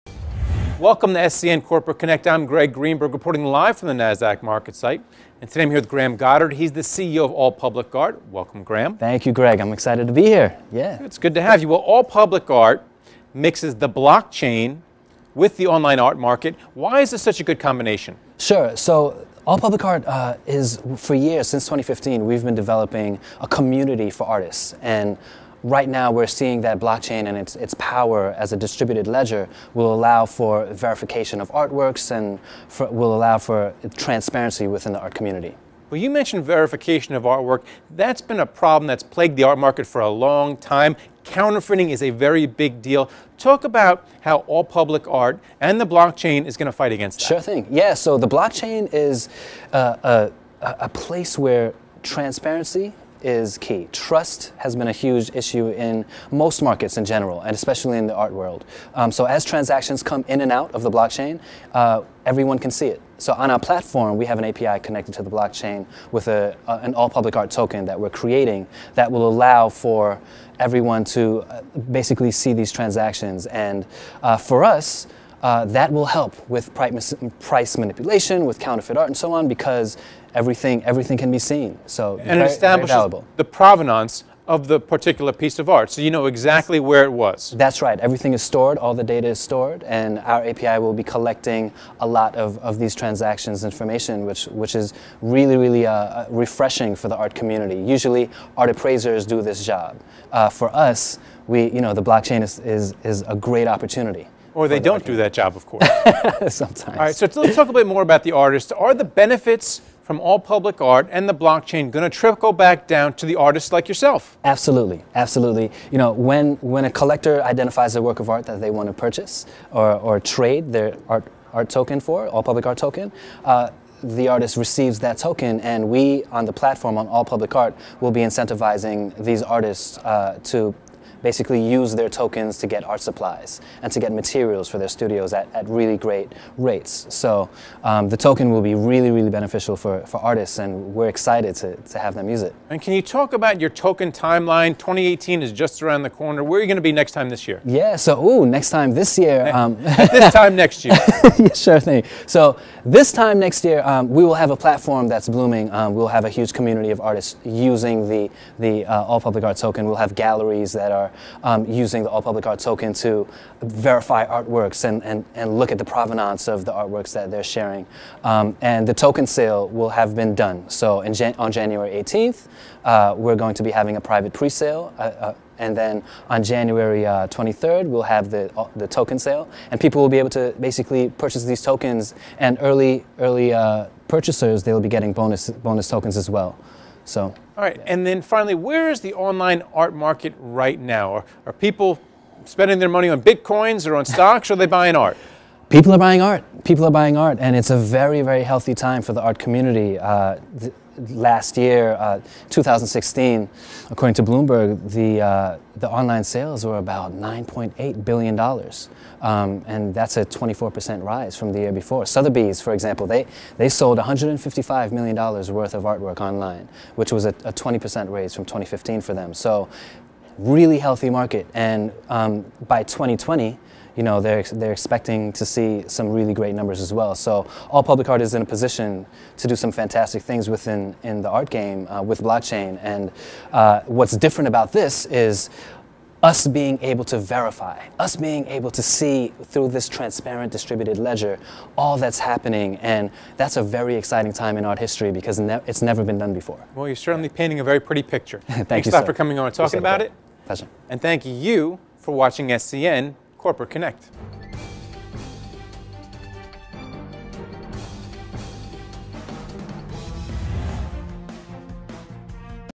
All Public Art founder interview Graham Goddard at NASDAQ
Graham Goddard, founder & CEO of ALL PUBLIC ART, explains how the company is merging Art and Blockchain technology with the launch of the AllPublicArt token.

This massive endeavor will result in the empowerment of artists, art collectors, art institutions and art galleries by helping them to securely store the provenance of artwork, verify their identity and confirm the authenticity of the artworks, while contributing to the minimization of counterfeit art, fraud, collusion and market manipulation.

With the implementation of blockchain technology, ALL PUBLIC ART can impact the art market and contribute to art history in a way that has NEVER been done before in the art world on a massive scale. This is a unique time in history for technology, and the ALL PUBLIC ART team and we feel privileged to have the responsibility of contributing to art history.

Filmed at NASDAQ for SCN Corporate Connect's television show.